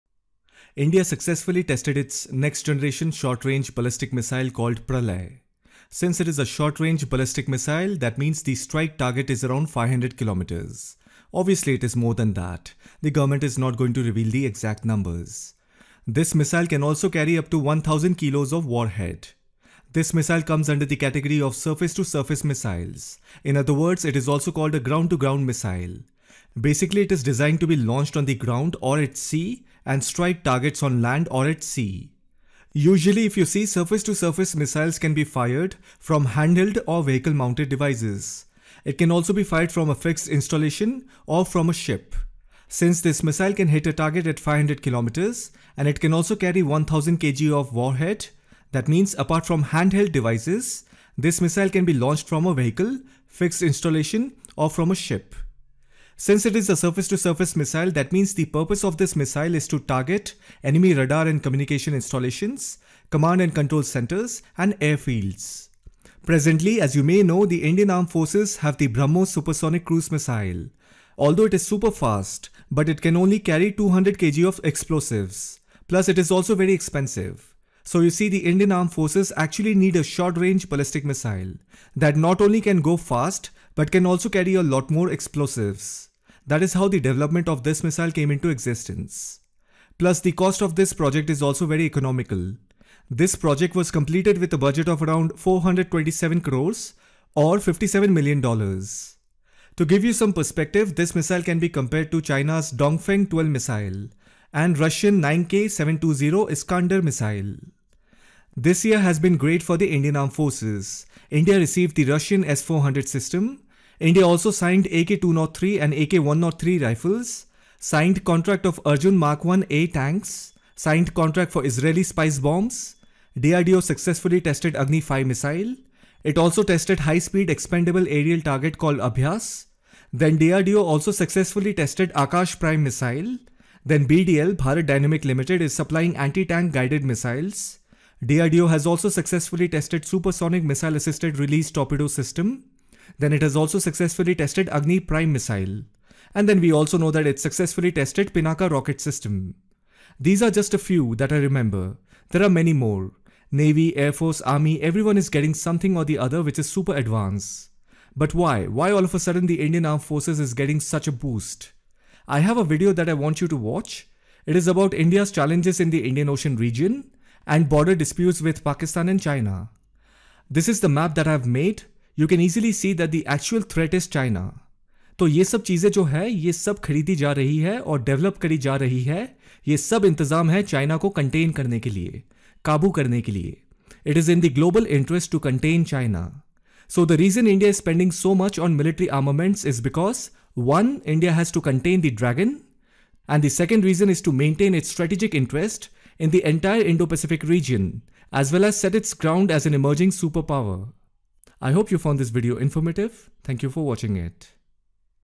Pralay missile successfully tested | Why India is boosting its military power | Indian Defence Deals
India successfully tested Pralay missile which is a short-range ballistic missile. Why India is boosting its military power? Indian Defence Deals 2020-21.